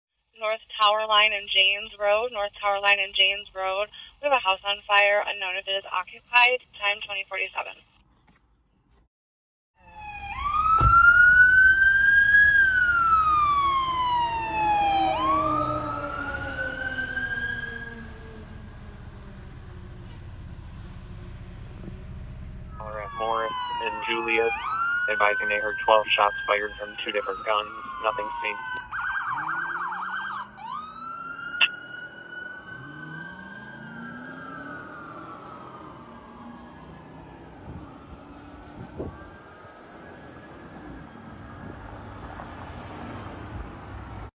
Saginaw MI FD E-2 Responding (COMMERCIAL FIRE ALARM)
Saginaw MI City Fire Department Engine 2 responding on Congress Avenue to a BOX 5 commercial fire alarm at Thompson Middle School on Court Street on Saginaw's West Side. Nothing visible upon first due Engine 2 arrival. All companies cleared.

Call: Commercial Fire Alarm. 5/13/24

🔴Saginaw Michigan City Fire Department
Station 2
Engine 2